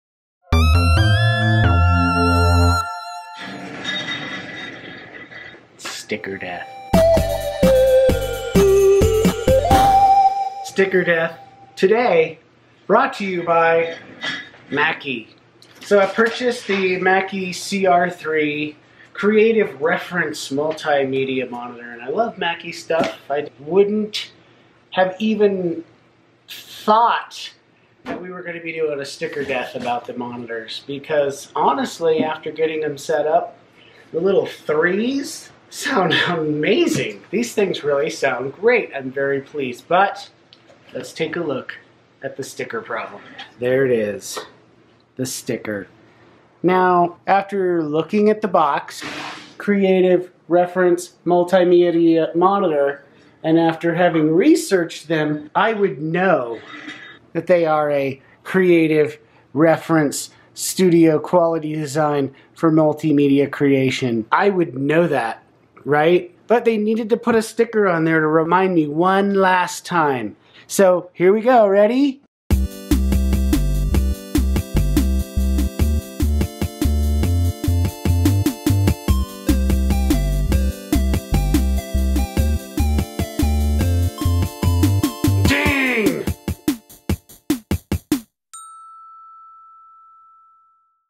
Sticker Death - Mackie CR3
Shame on you Mackie  for putting a goopy, useless  sticker on the CR3 Studio Monitor.